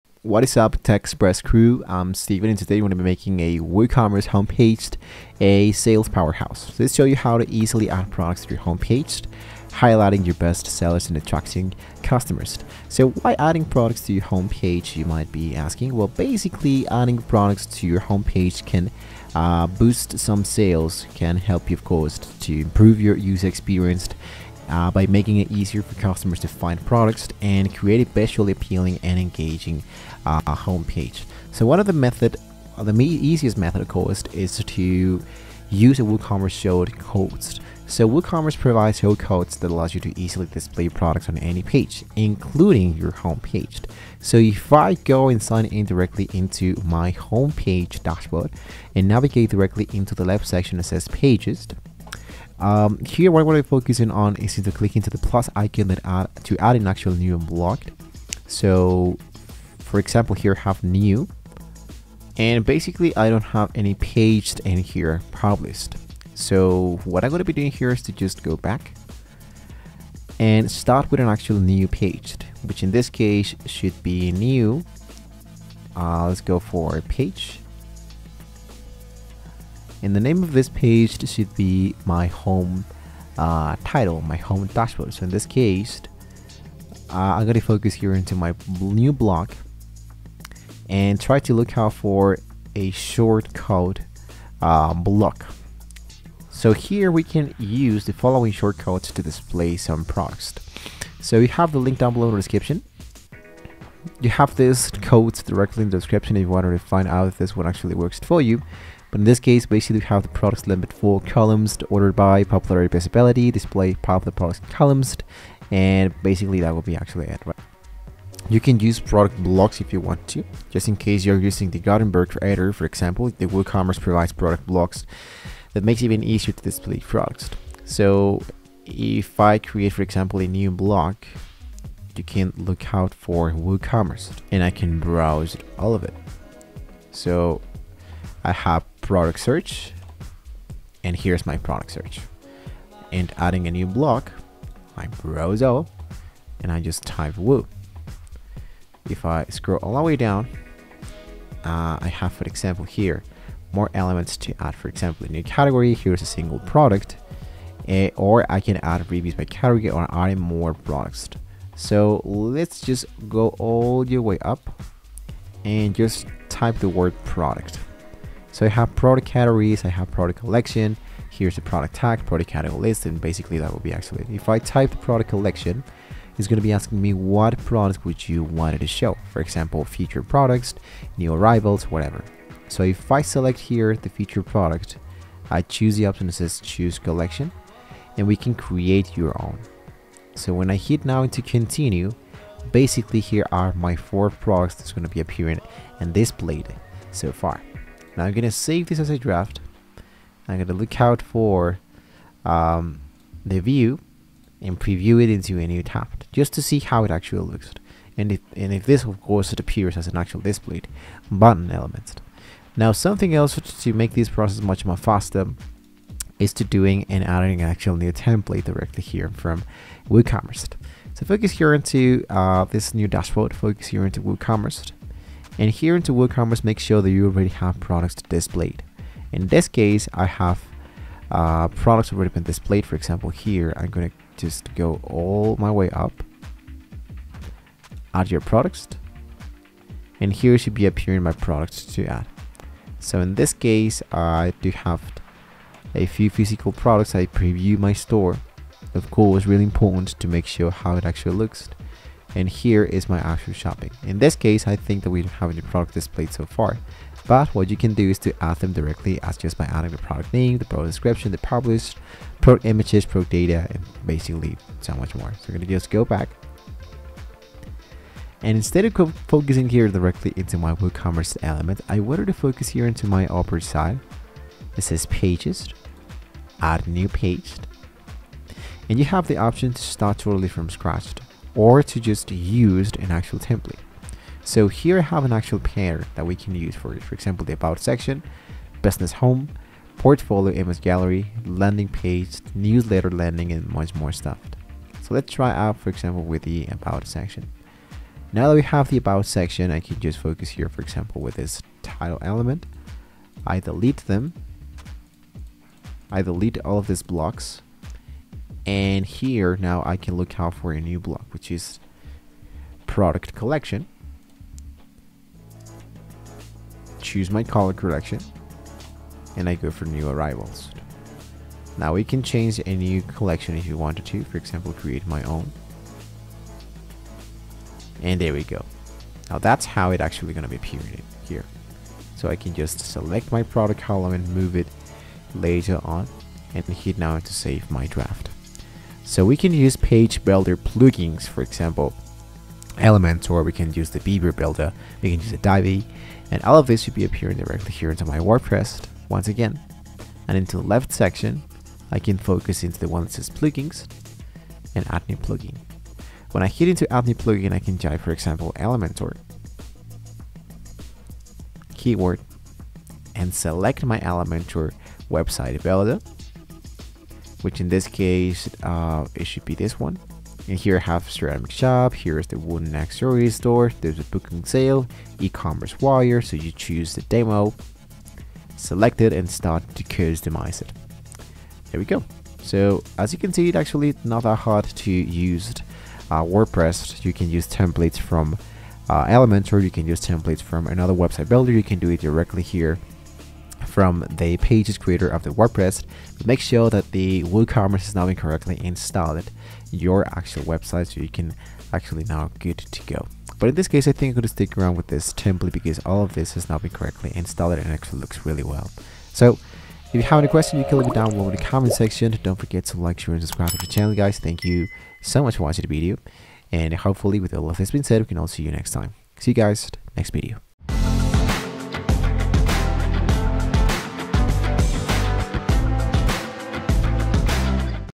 How To Add Products On Your Homepage With WooCommerce (2026)
Adding products to your homepage with WooCommerce is a straightforward process that enhances your online store's visibility. Start by accessing your WordPress dashboard and navigating to the "Appearance" section. Select "Widgets" or use a page builder, depending on your theme. Utilize the WooCommerce product widget or shortcode to display featured products, categories, or bestsellers.

Customize the layout and design to align with your brand aesthetics. Ensure that products are properly categorized and tagged for optimal user experience. After making adjustments, preview the changes before publishing to confirm that everything appears as intended. This method effectively drives customer engagement and boosts sales.